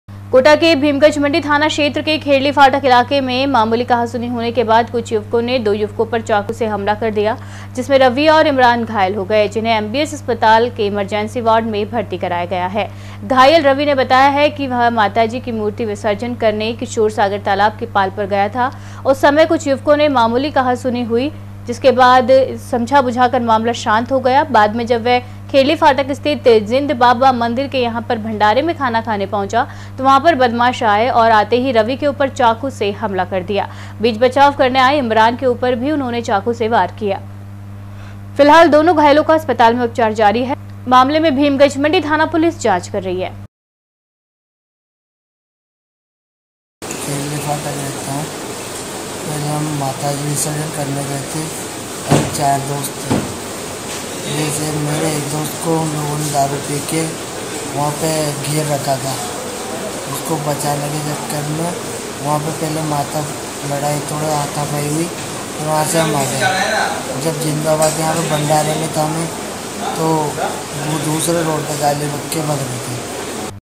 News 06 oct 2022 @ कोटा- भीमगंज मंडी इलाके में चले चाकू घटना में दो युवक घायल
कोटा- भीमगंज मंडी इलाके में चले चाकू घटना में दो युवक घायल